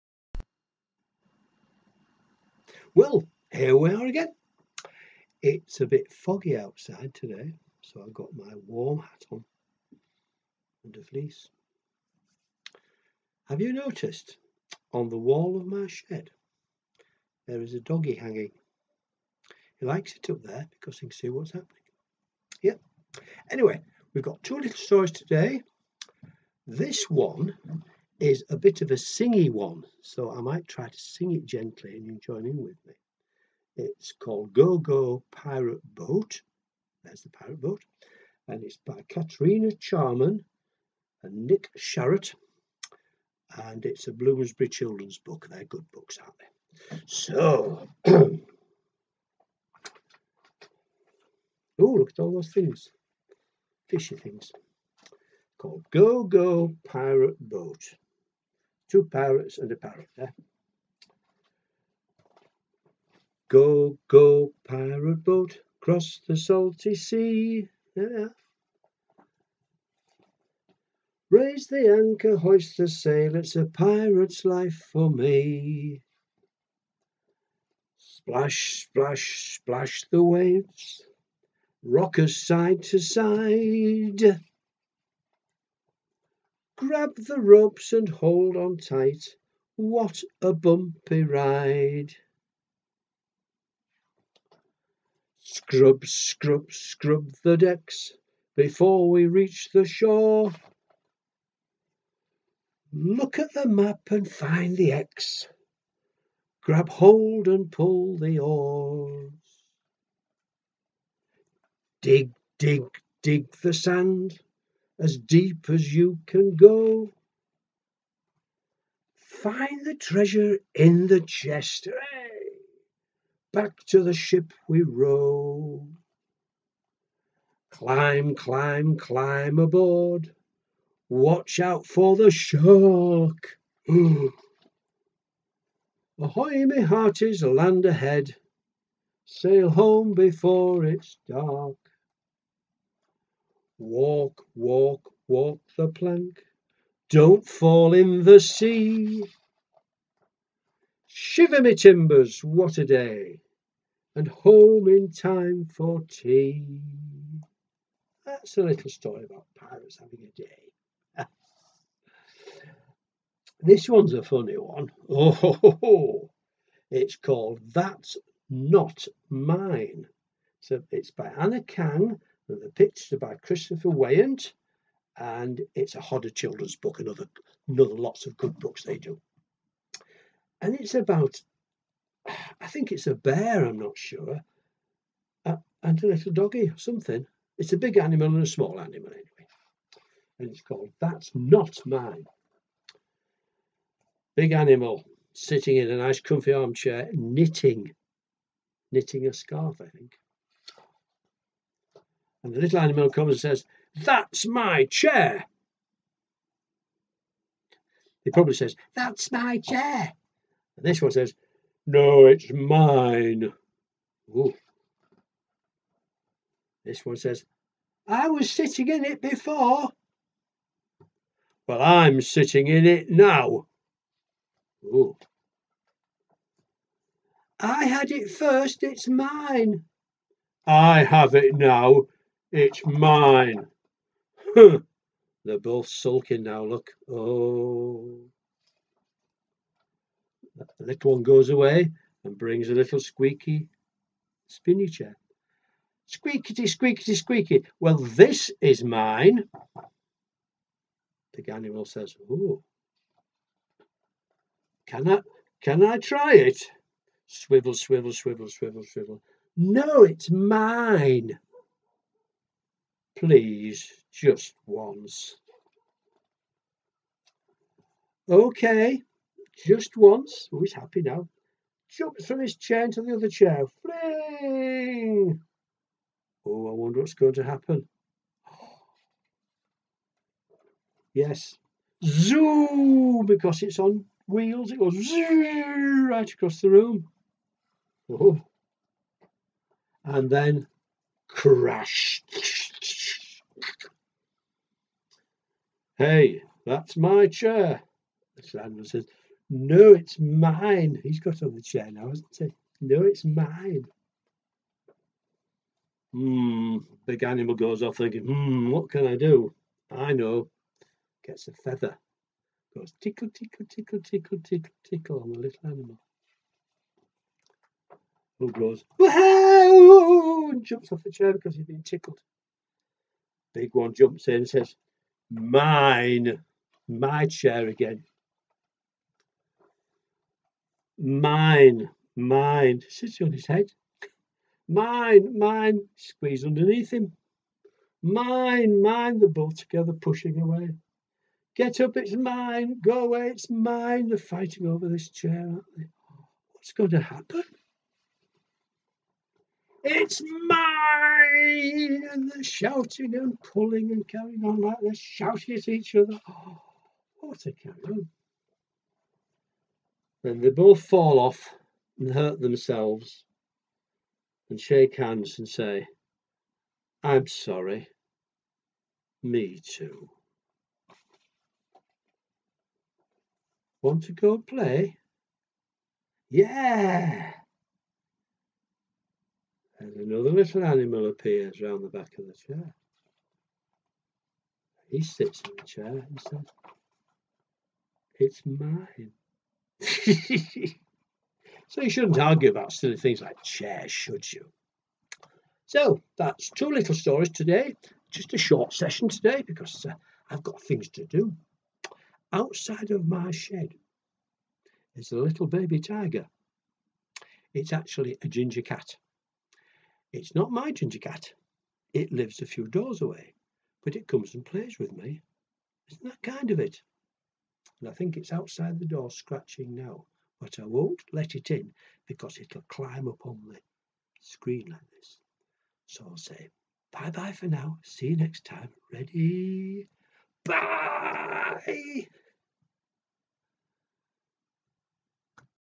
Storytime with Robert at Filey No15 - Pirate Boat & My Chair
Go, Go, Pirate Boat, by Katrina Charman and Nick Sharratt (pub Bloomsbury) is a happy little story that you can sing along with.  Then we have That's Not Mine by Anna Kang & Christopher Weyant (pub Hodder) - a chaotic bit of silliness that tells you why you shouldn't argue about things that don't matter.